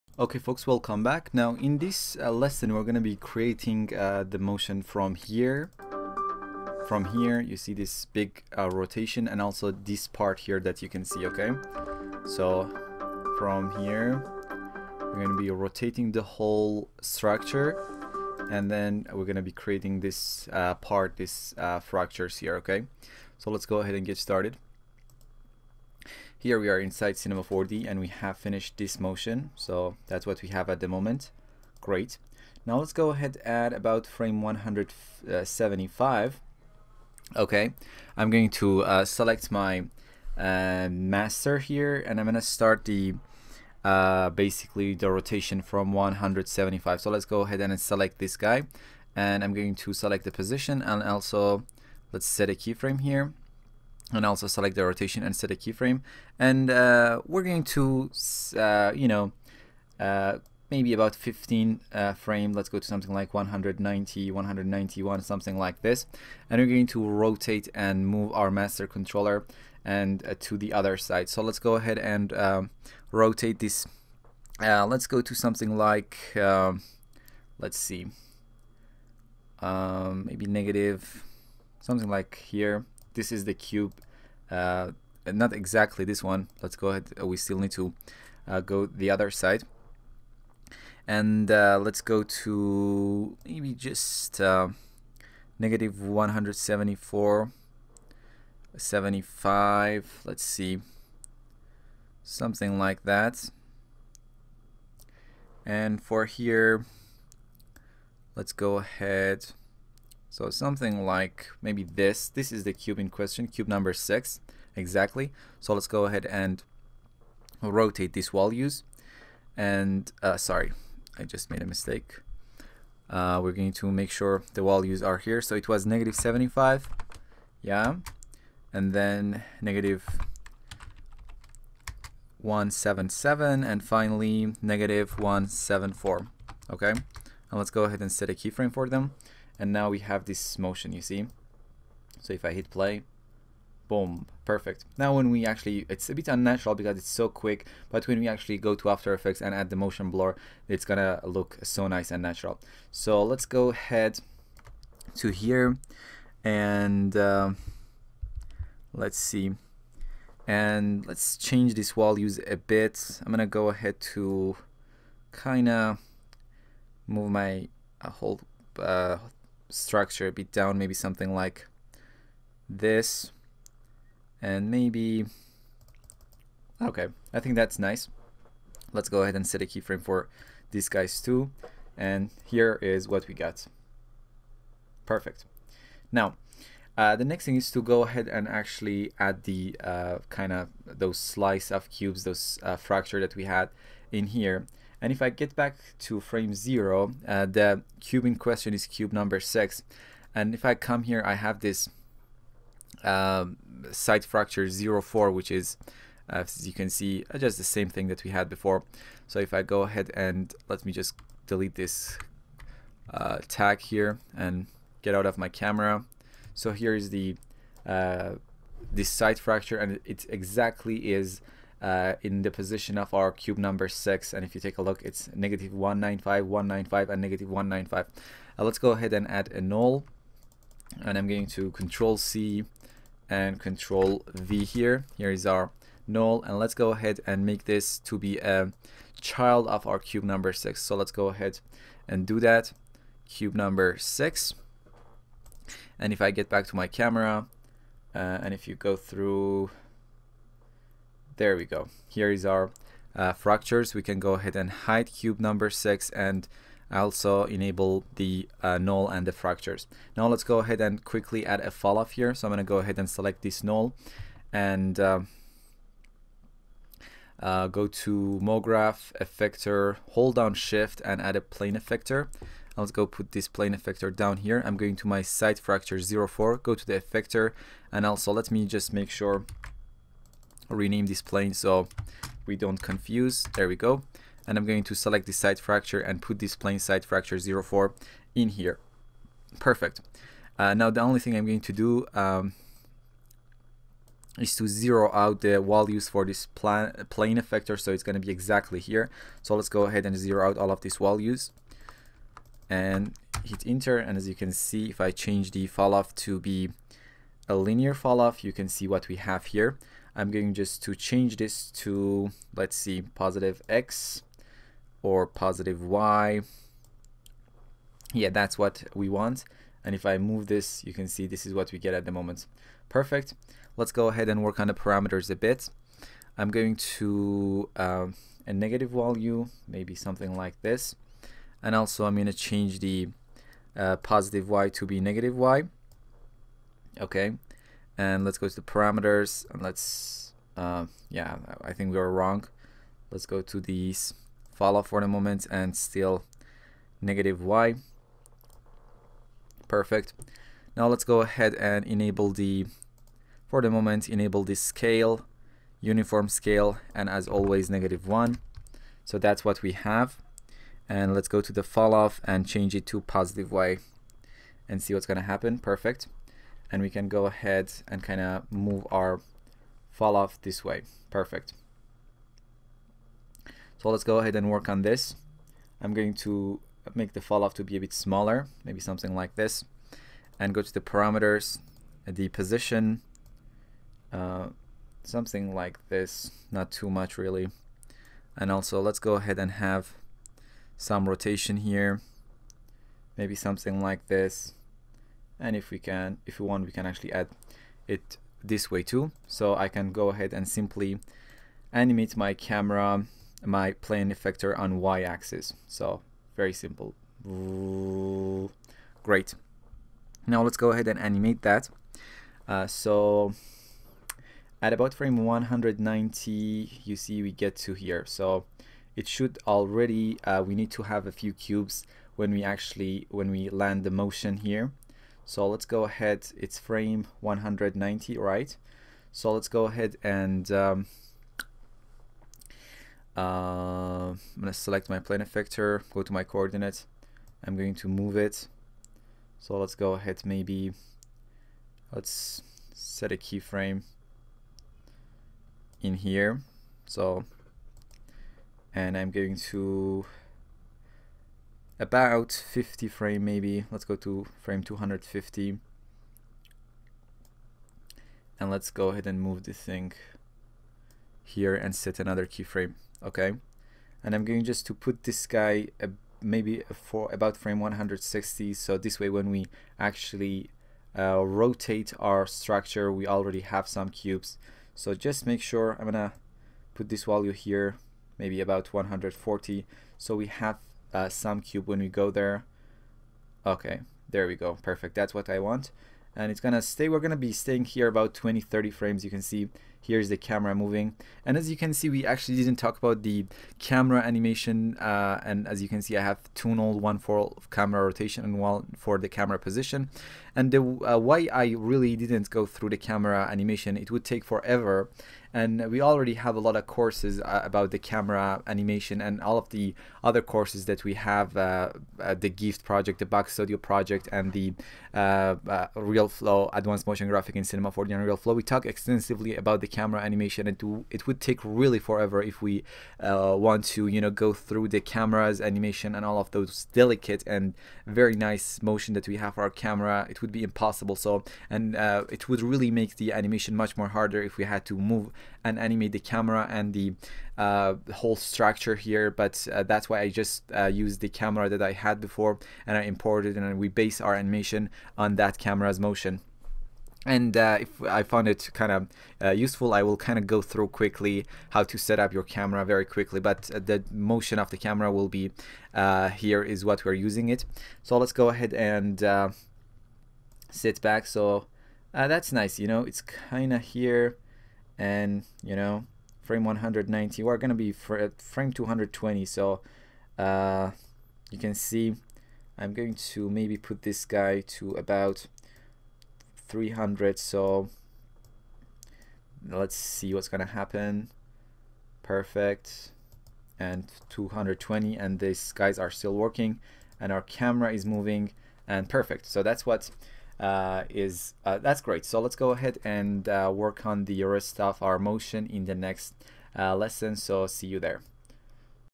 Creating a Stunning 3D Motion Design in Cinema 4d and After Effects | ( 10 of 18 )
In this tutorials in almost 4 Hours , we go through the process of creating advanced 3d motion graphics in Cinema 4d and After Effects .This is another one of our project-based tutorial in which we take one motion graphic design and show you how to create that motion graphic from scratch .

This animation is based on a motion design by Box Studio